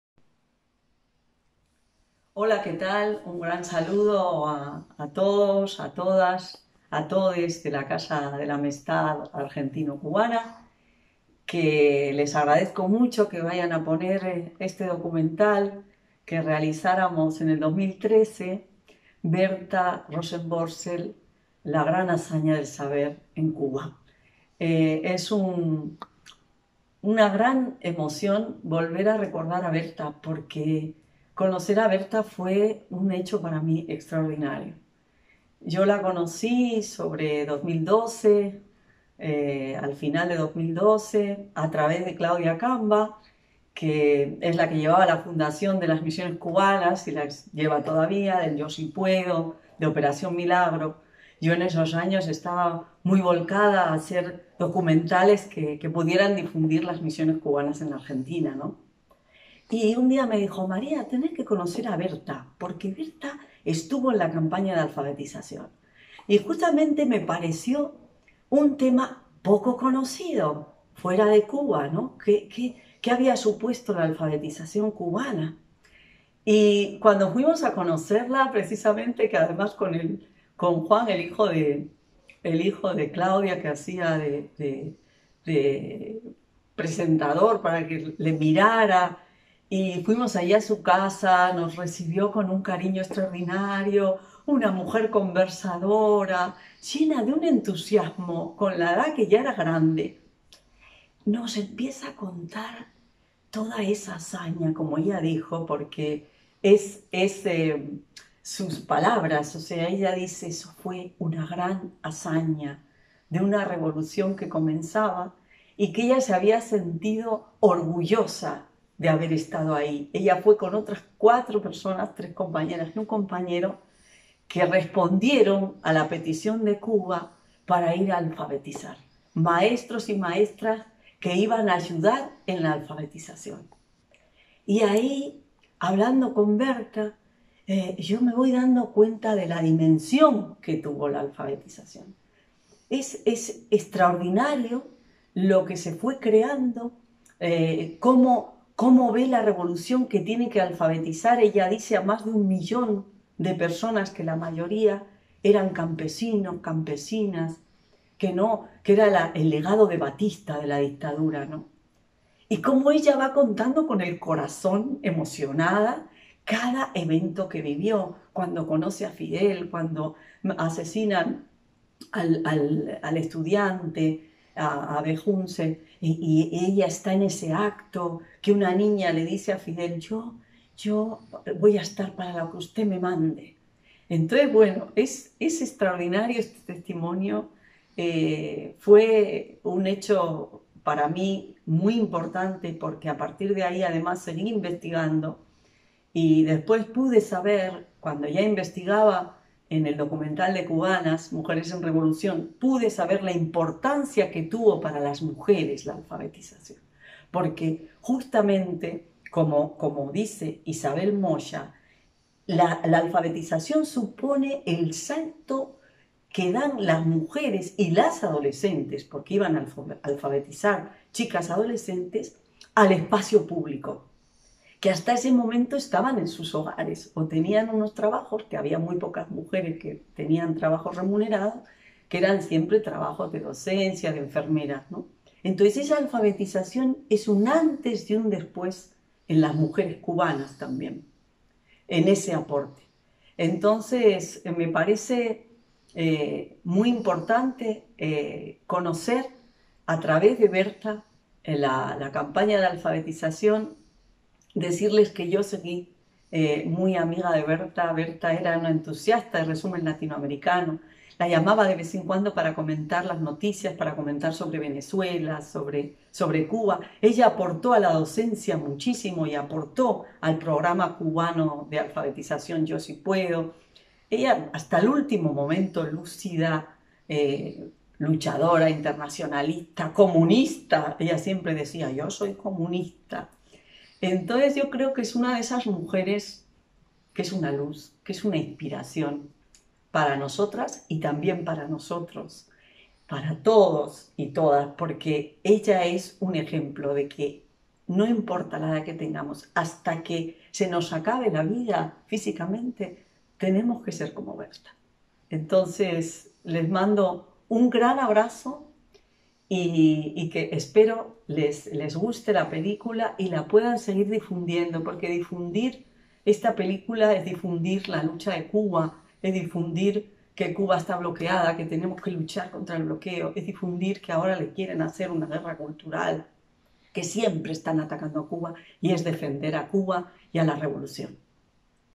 Presentación de Berta Rosenvorzel_ la hazaña del saber en Cuba
Documentalista Maria Torrellas recuerda a Berta Rosenvorzel.